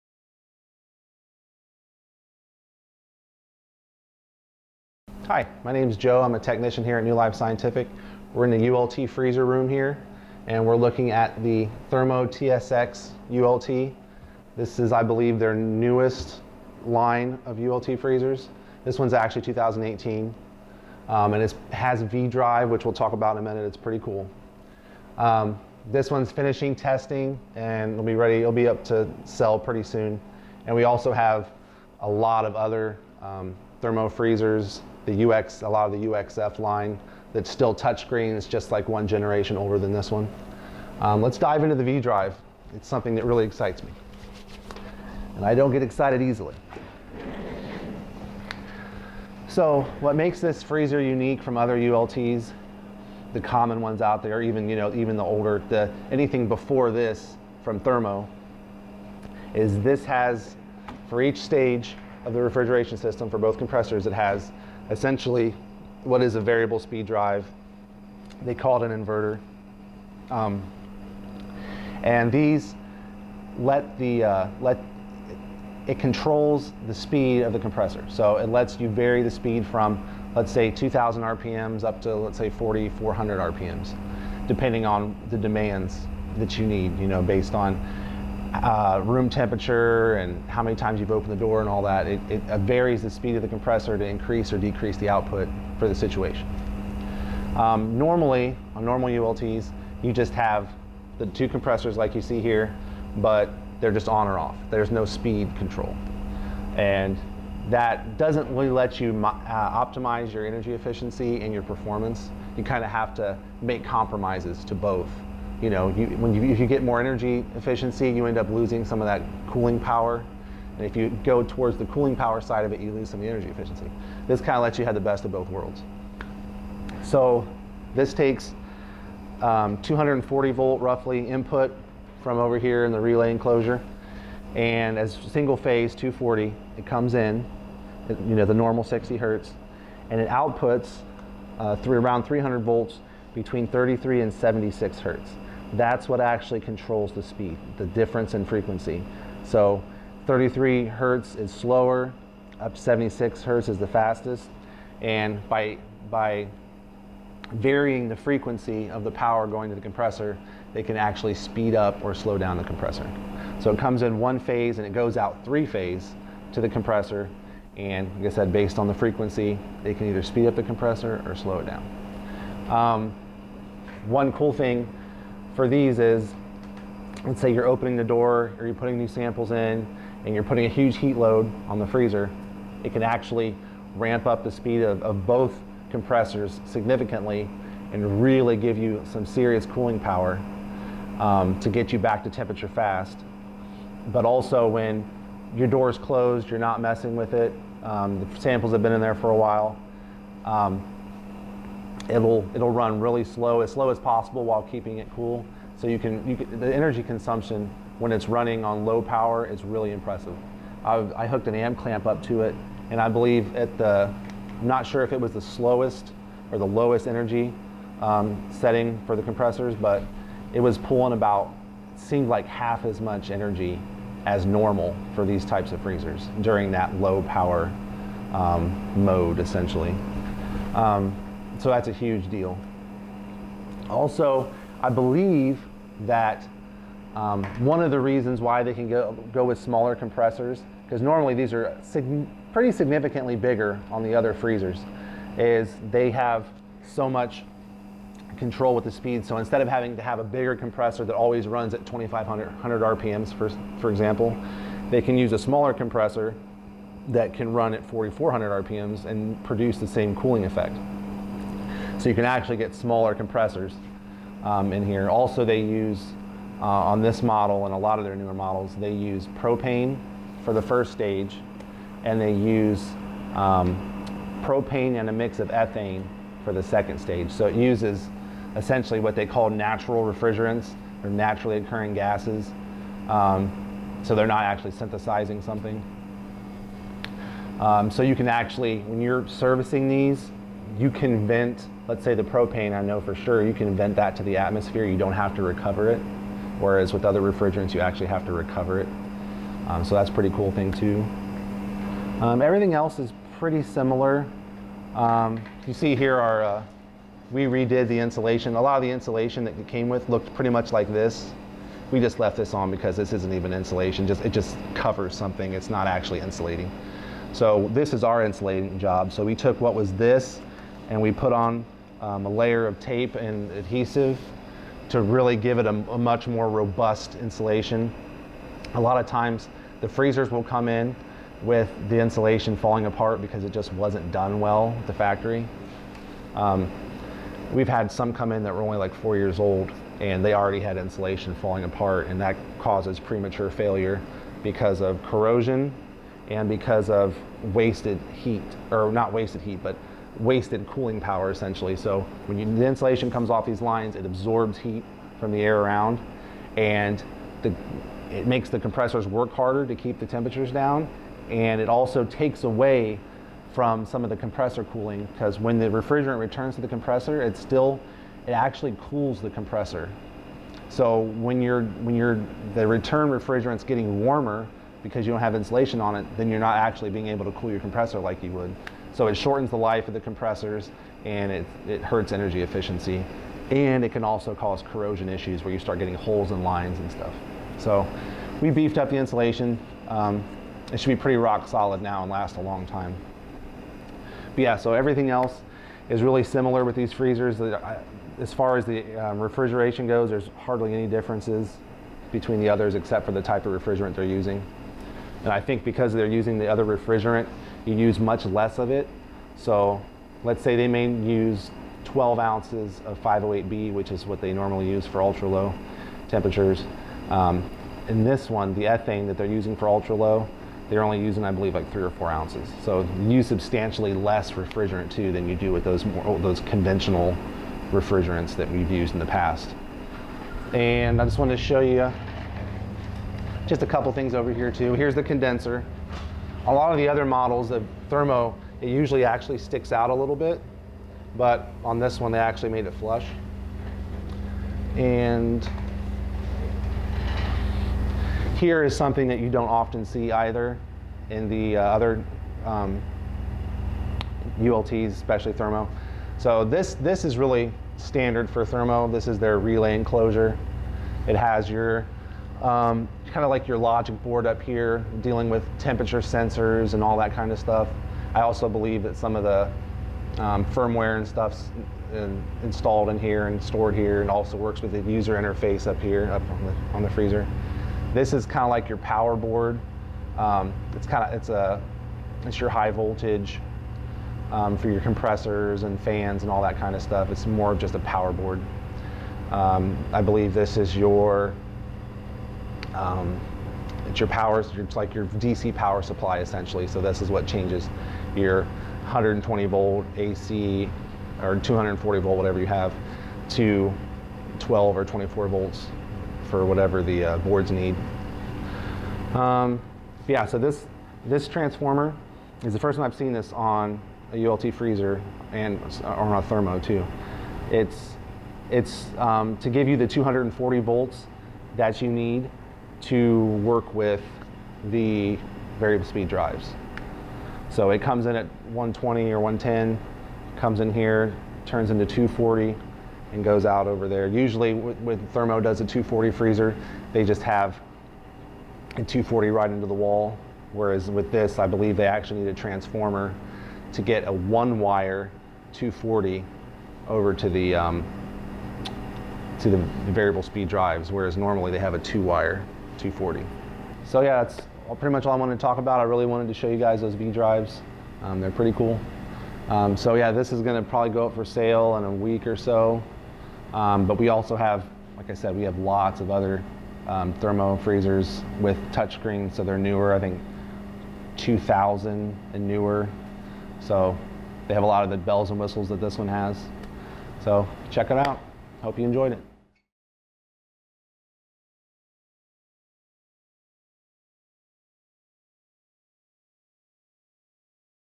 What is the V-drive Technology at the core of Thermo TSX Series ULT Freezers?
In this video, one of our technicians shows off the V-drive technology (variable speed drive) inside the TSX Series (ULT) ultra-low temperature freezers from Thermo Scientific. If you are looking to purchase a ULT freezer, then we may have one available that will meet your needs